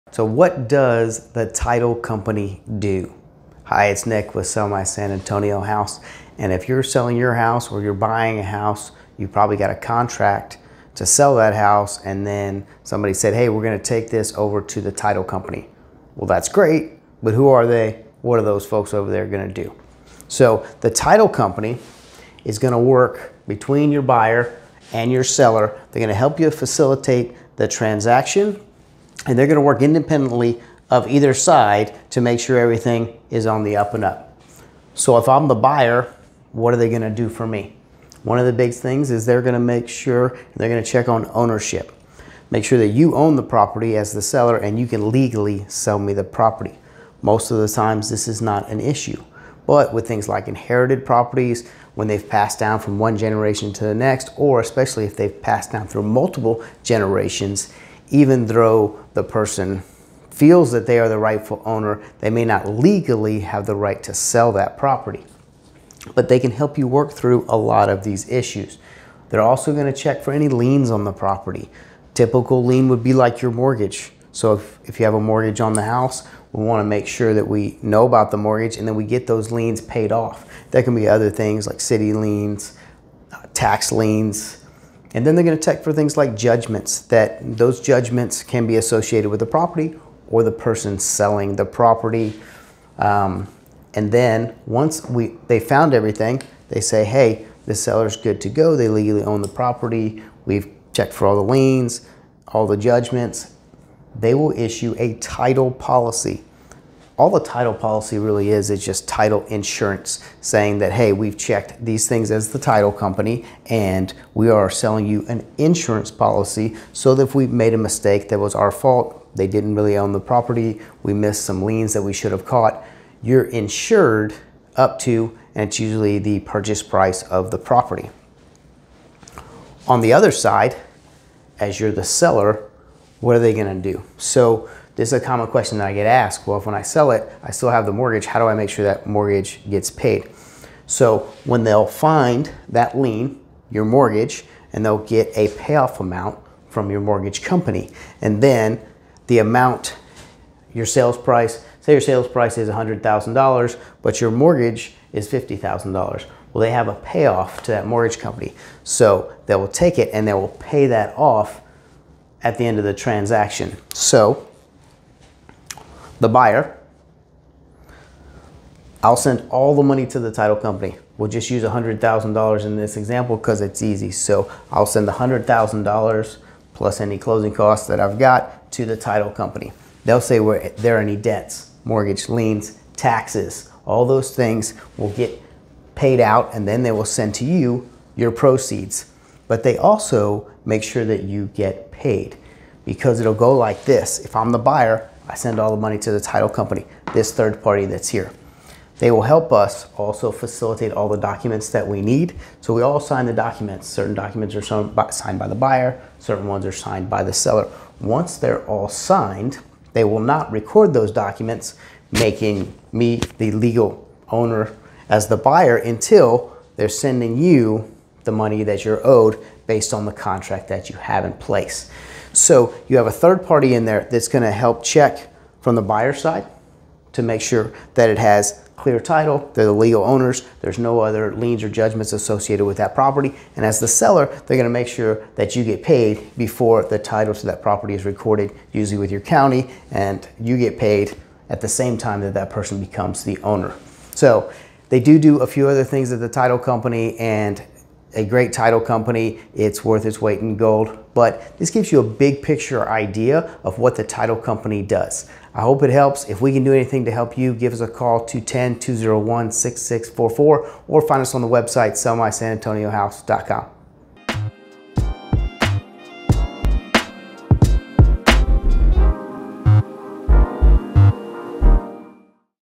What Does The Title Company Do?
The title company helps facilitate the purchase and sale of real estate. Some of the things they do help protect the buyer and some of the things they do help protect the seller. Learn what the title company does and why it is important to use a title company when you are buying or selling property!